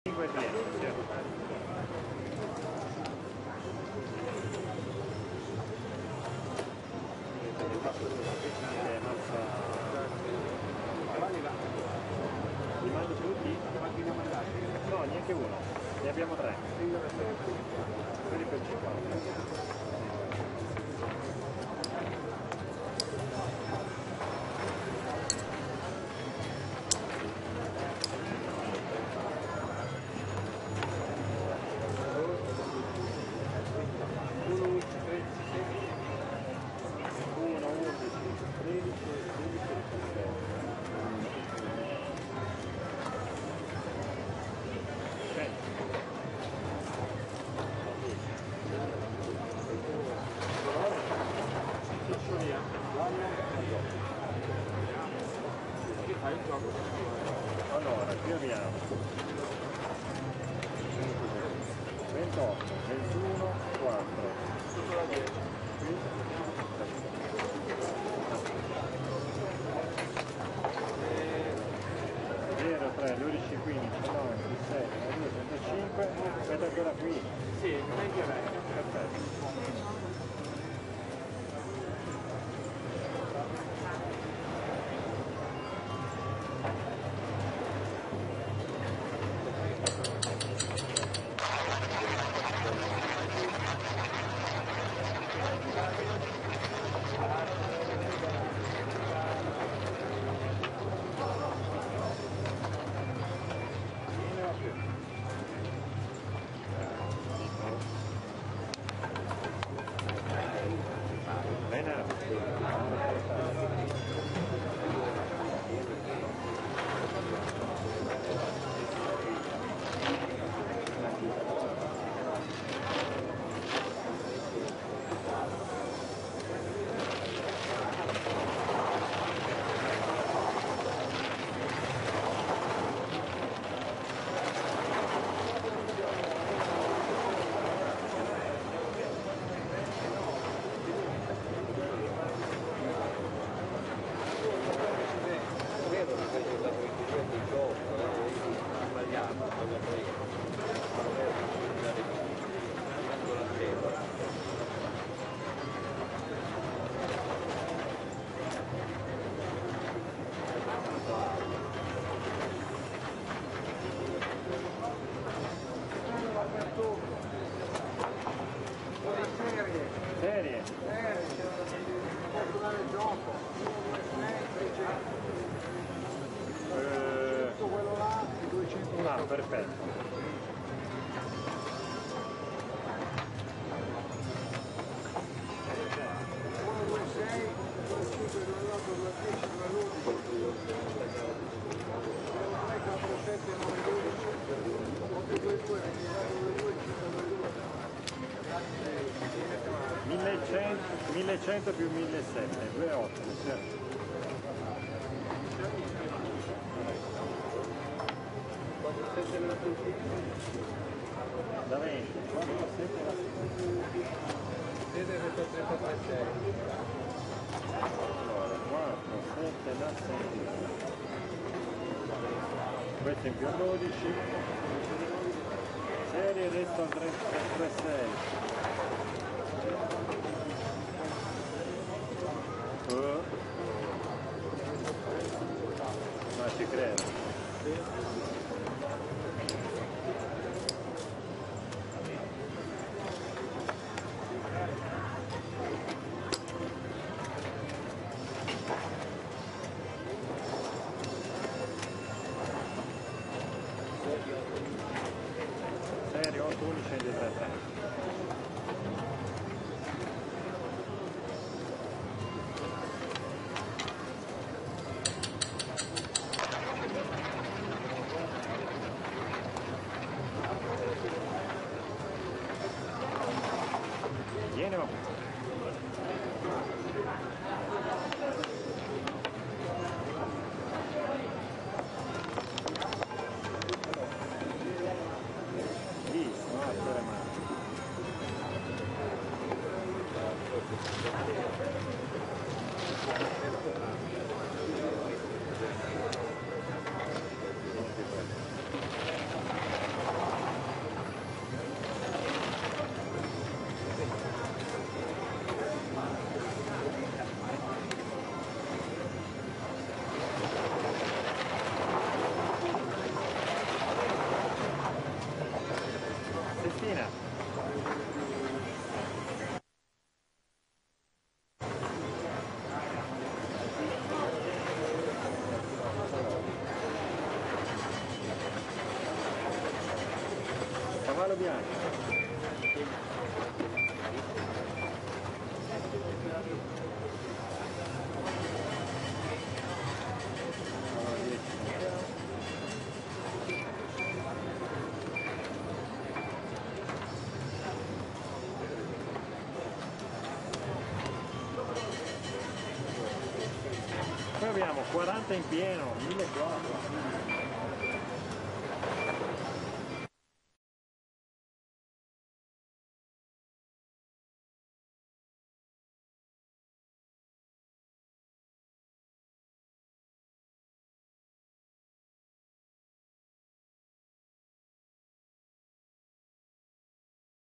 Roulette Professional Saint Vincent - Italie
Découvrez la vidéo de la Authentic Roulette Professional filmée en direct du casino Saint-Vincent en Italie. Roulette en ligne accessible aux internautes.